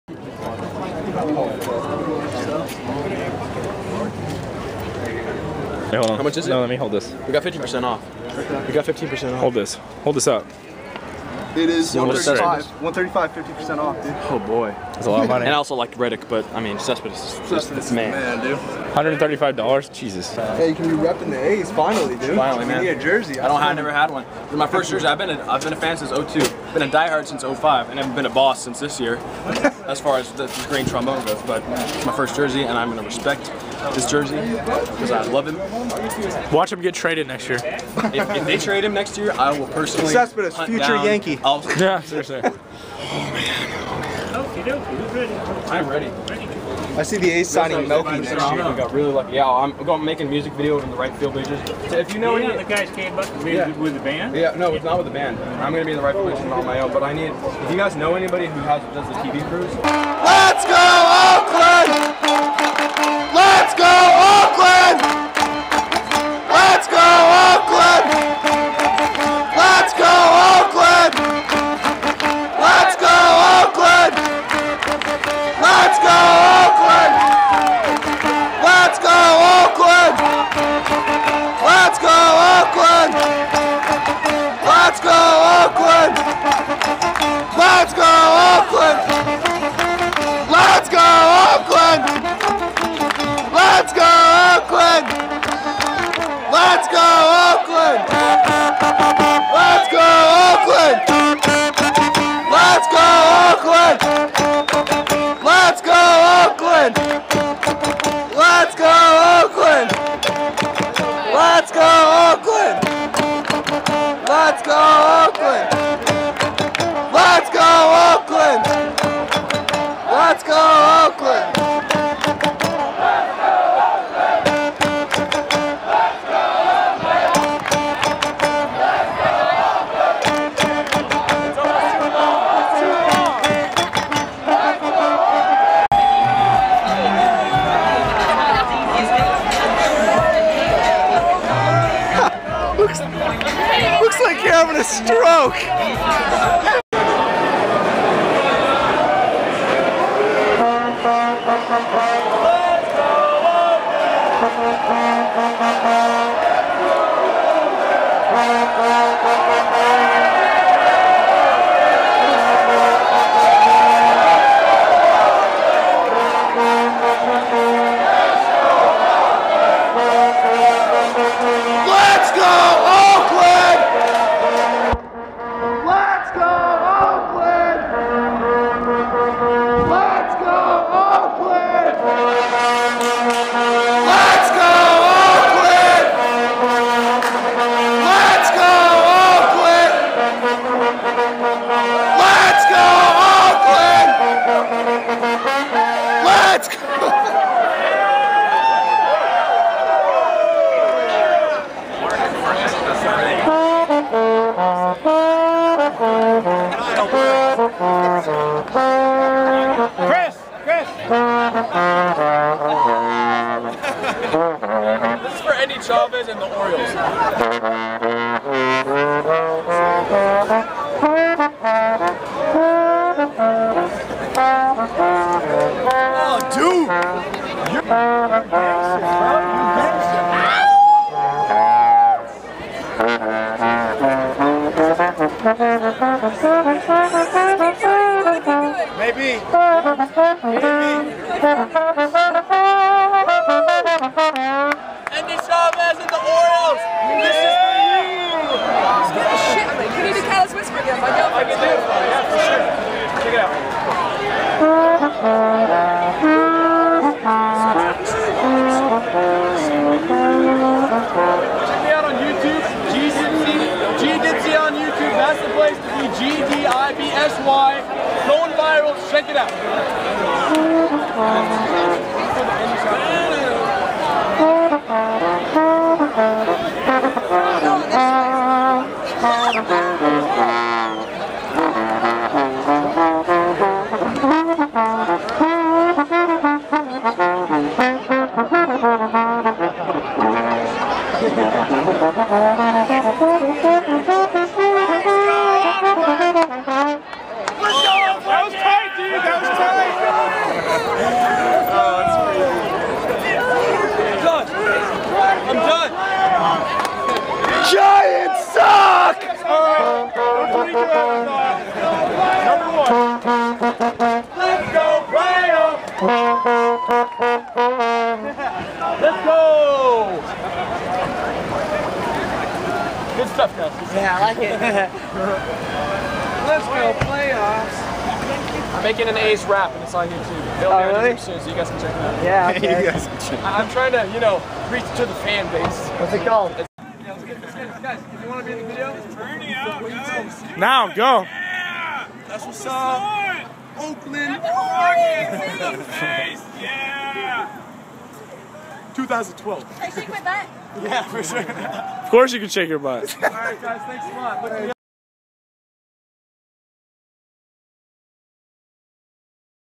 GDibsy - The Green Rally Trombone - Oakland Athletics Playoff Run 2012
I love this team. Oh and I didn't know that that guy at the store was referring to the drummers as the band! If so then I was definitely with them haha. I love the bleacher fans because they are chill and great fans. And with this video, I'm not trying to take credit for the awesome fans in Oakland; I am simply blasting my trombone with pride and helping keep people on tempo. I am not a bandwagon fan - just know that. I have been with this team hardcore since '05, through the 2007, '08, '09, '10, and '11 seasons when we weren't a winning team. When I bought that jersey, I had $300 left in my bank accounts after a summer of spending and preparing my songs/debut music album. Now, it's time to shine - both for me and the A's. I live for this team. I bleed green and gold. GET ON THE HYPE!!! LET'S GOO OAAAKLAND!